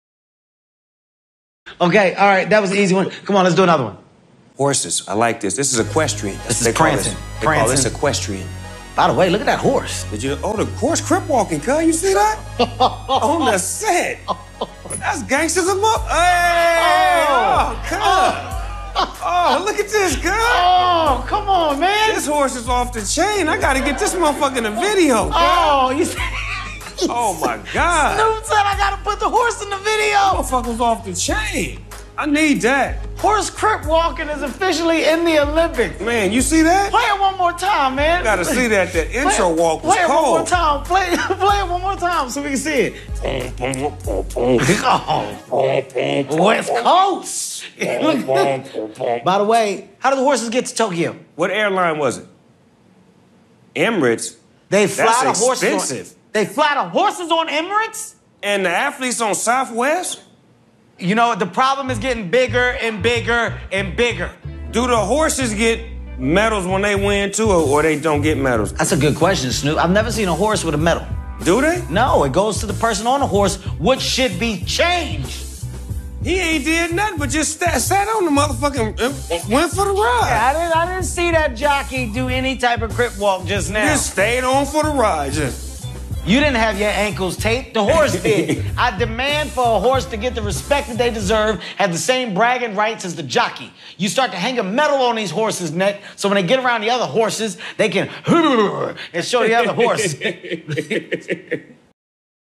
Snoop & Kevin Hart Commentate on Olympics Equestrian Event - "The Horse Crip Walking Cuz"
Snoop is blown away by the crip walking horse and his cold ass dance. Followed up by some corny ass Kevin Hart joke that I apologize I couldn't edit out this mfn video